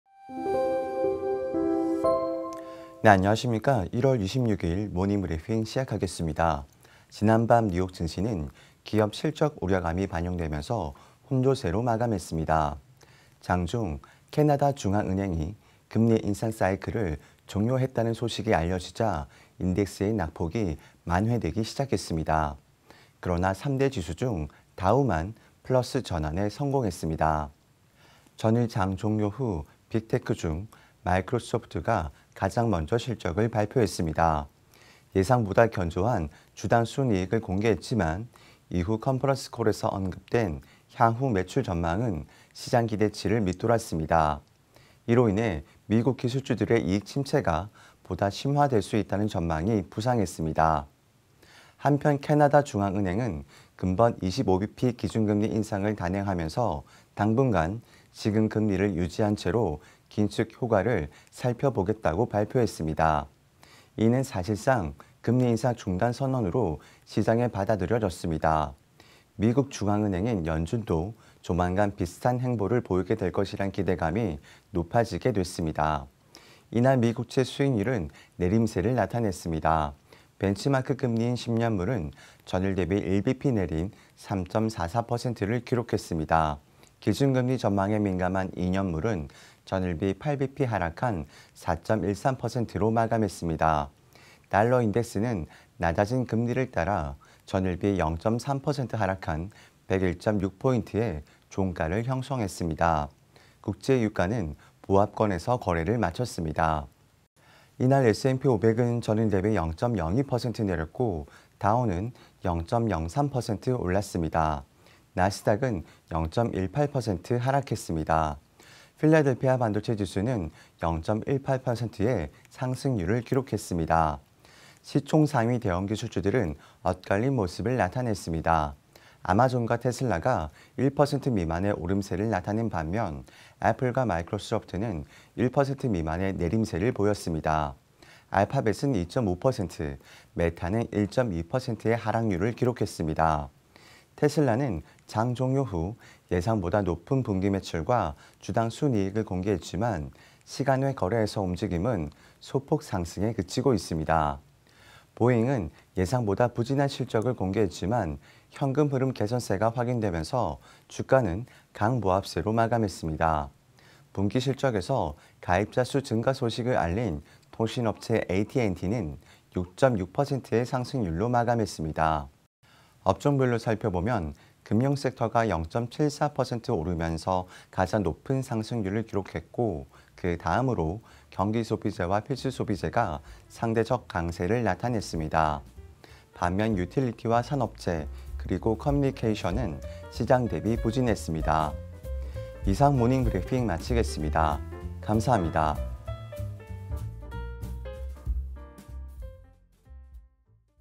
모닝 브리핑 / 2023년 01월 26일(목)
삼성증권이 전하는 모닝 브리핑

00:07 미국 시장 주요 이슈

01:47 미국 시장 주요 지수

02:53  미국 시장 업종 등락